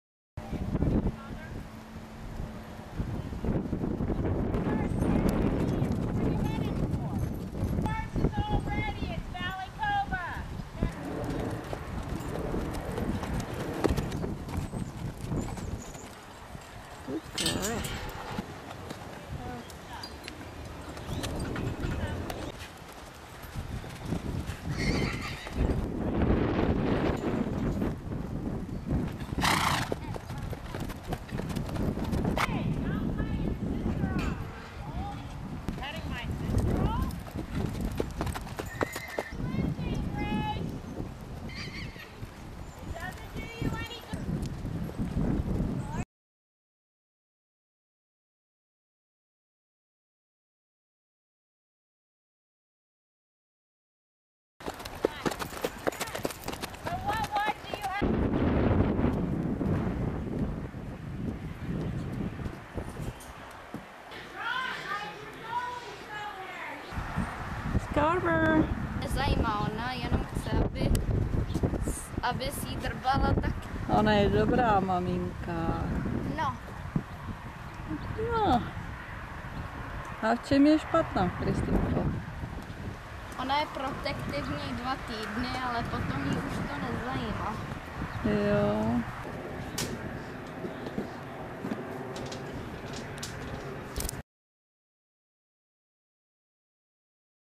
The Mounted Guardian Angel (Anděl strážný v sedle)
My granddaughter Kristyna in her new role of a mounted guardian angel and also with her foal Scarborough. (Vnučka Kristýna ve své nové úloze jízdního anděla strážného a taky se svým hříbětem Scarborough.)